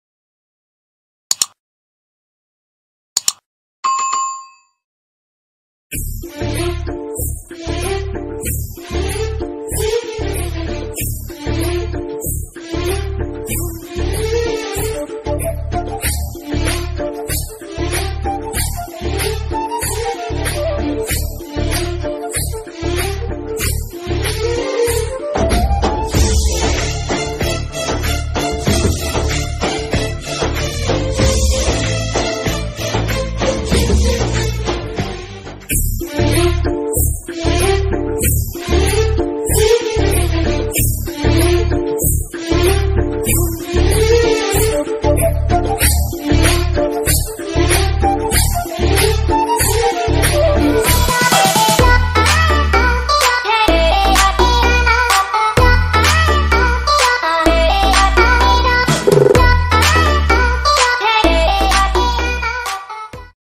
Amayra k cute photo betuu ki cute si hassi.7th month ki hogyi h wish nhi kroge?AmayraDhakadOfficial
Amayra k cute se photos with his cute smile😊😊... 7th month ki hogyi.. wish nhi kroge?? Betuu 💖 cutebaby 😇😇😇 funday 😅😅